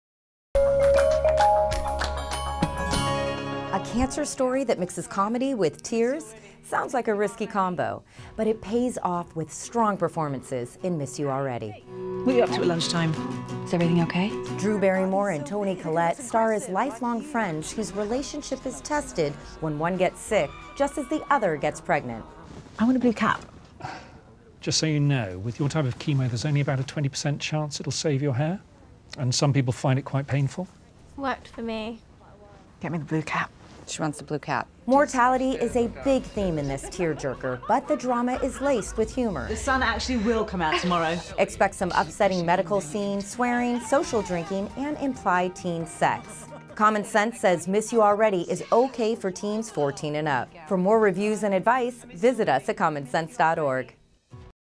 Miss You Already Review
Our Stress-Free App for Finding The Best Kids’ Entertainment Drew Barrymore and Toni Collette star as lifelong friends whose lives take an unexpected turn when one is diagnosed with cancer.

Director: Catherine Hardwicke
Cast: Toni Collette, Drew Barrymore
Studio: Embargo Films
MPAA Rating:  PG-13